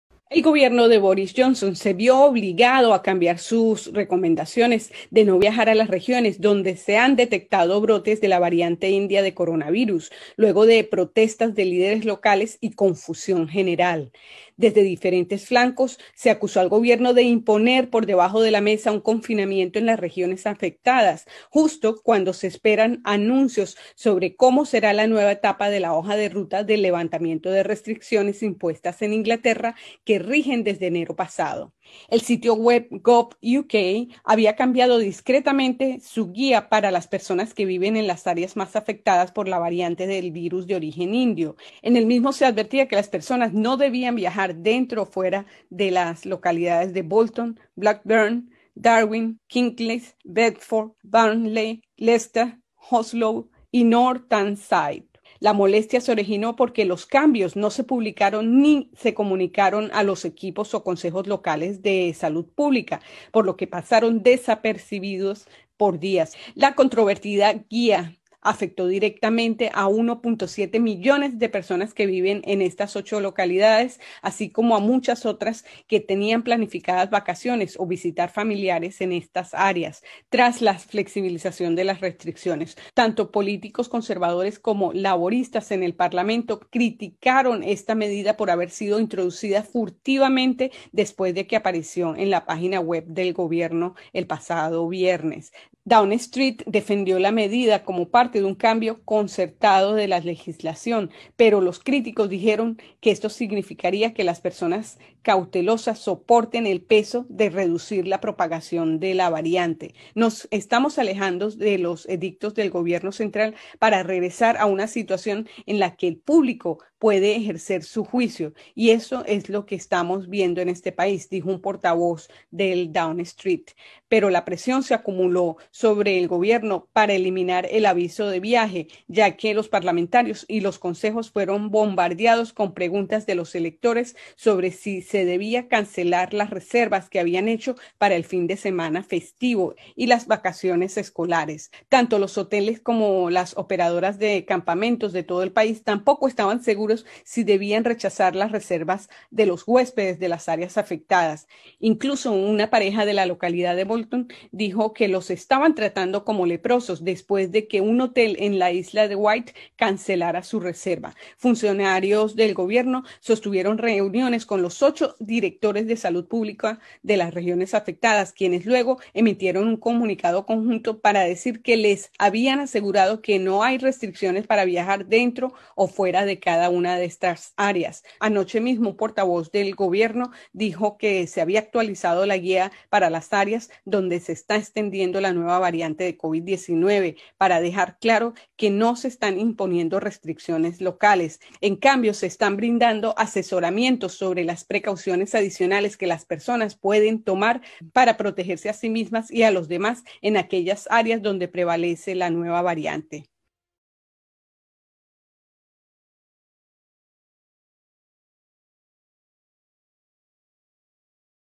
El Gobierno se retracta sobre los consejos de viaje locales
Gobierno británico da marcha atrás en los consejos de viaje locales a zonas afectadas por la variante india del Covid-19, después de que un cambio en las indicaciones provocara una gran confusión.